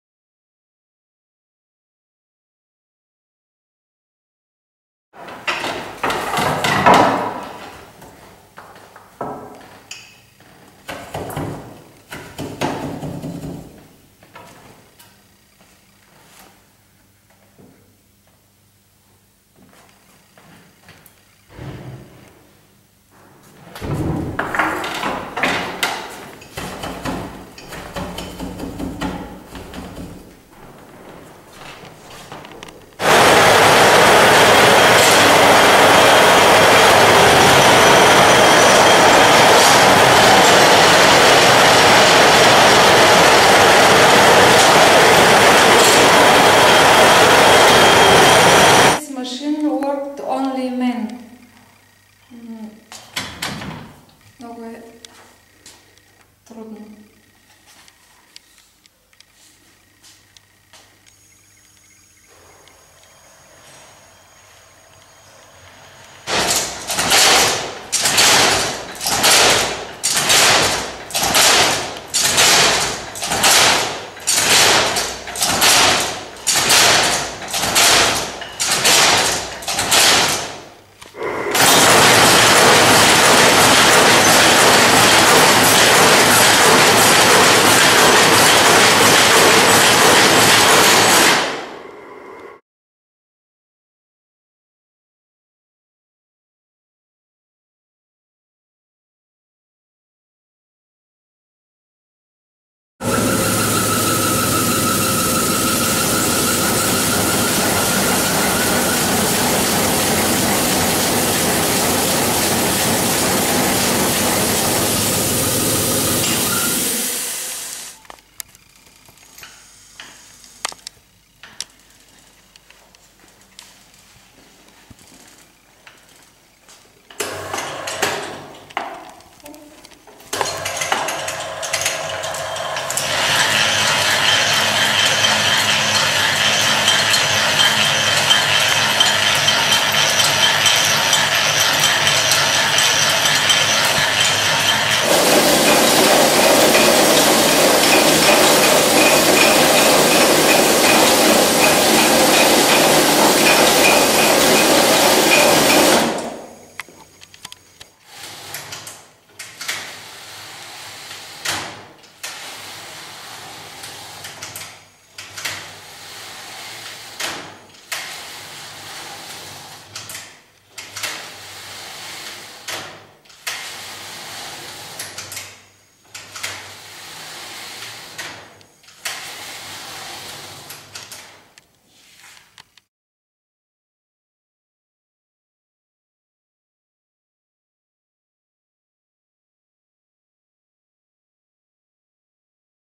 National Museum of Textile Industry
Demonstration Center at National Museum of Textile Industry, Sliven, Bulgaria